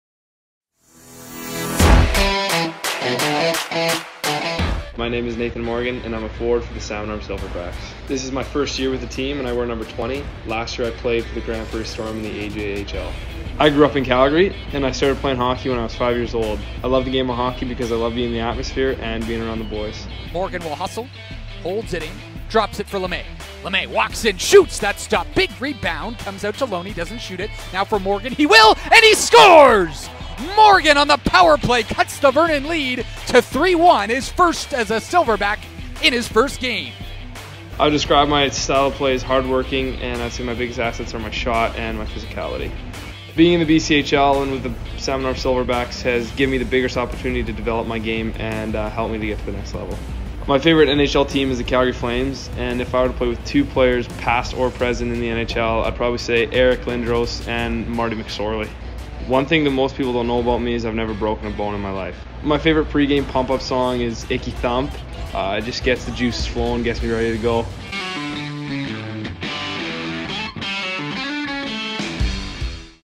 Meet the Silverbacks - Nathan Morgan
For the month of March, the Silverbacks will be doing a 'Meet the Players' series!

In this edition, we get to know hard-working winger Nathan Morgan!